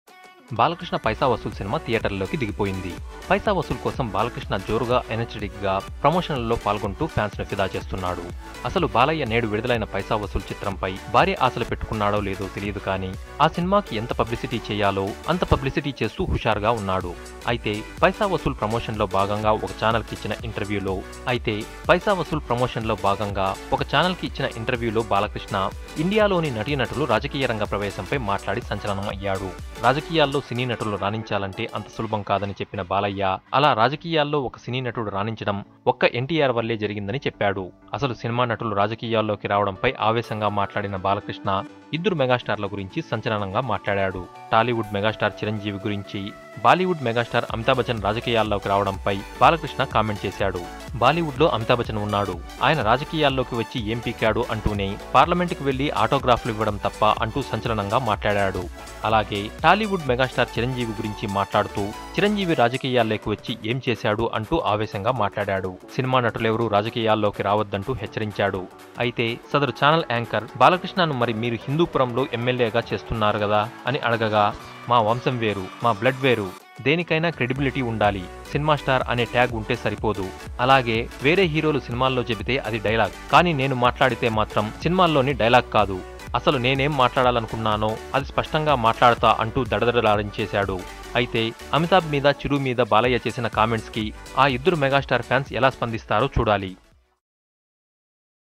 Balakrishna SHOCKING Comments on Chiranjeevi and Amitabh Bachchan | Celebs News | 70MM Telugu Movie
Balakrishna SHOCKING Comments on Chiranjeevi and Amitabh Bachchan. Check out the full details in this video on మెగా స్టార్స్ అమితాబ్, చిరులు ఏం పీకారంటున్న బాలయ్యసంచలన కామ్ంట్స్...! on 70MM Telugu Movie.